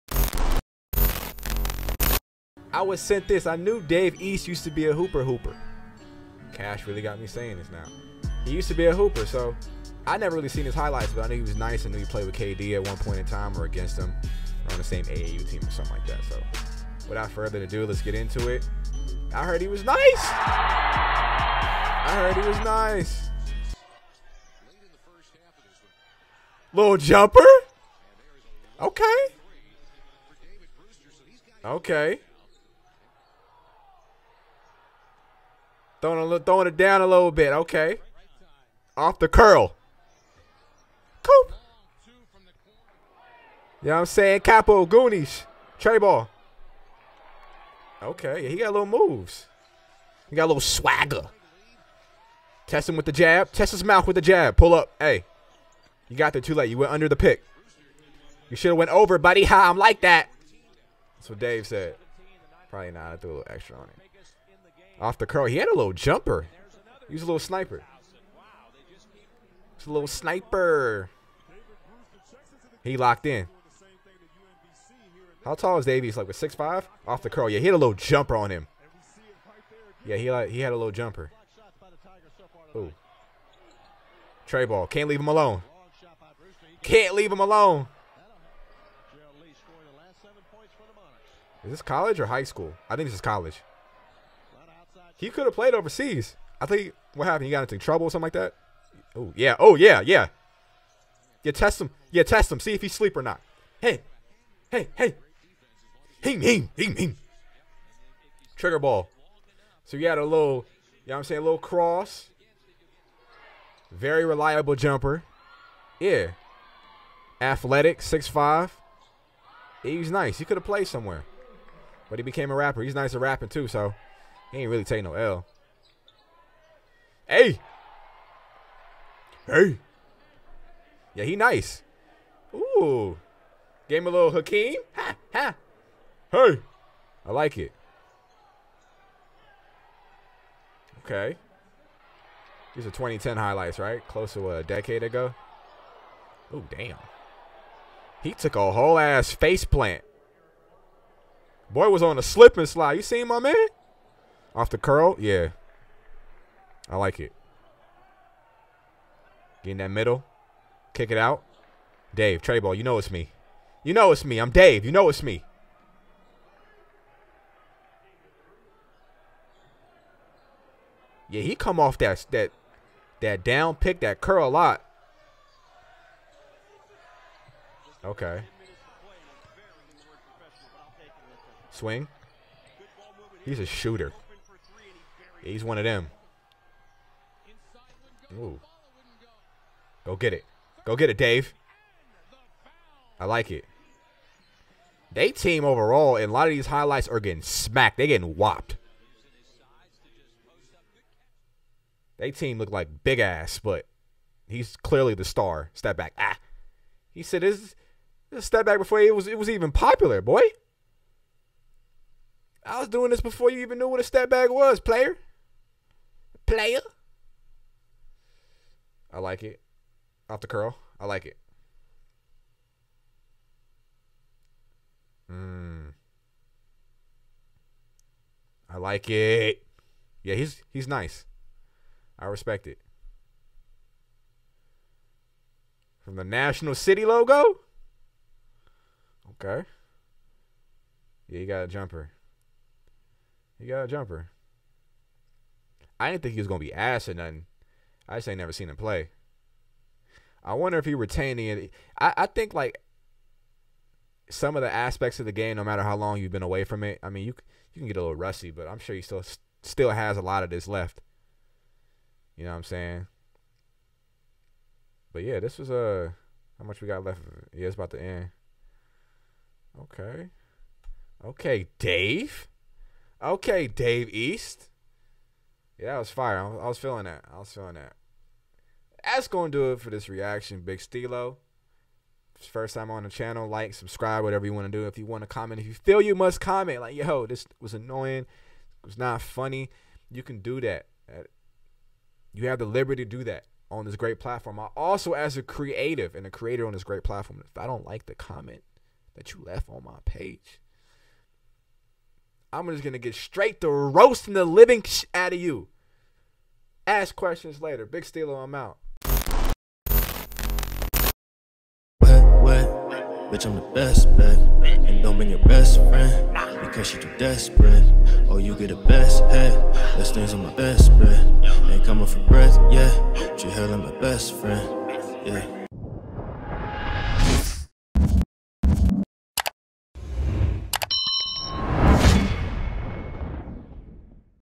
DAVE EAST COULD HAVE MADE IT TO THE NBA! | Dave East College Basketball Highlights REACTION
Do you think Dave East could have made it to the NBA?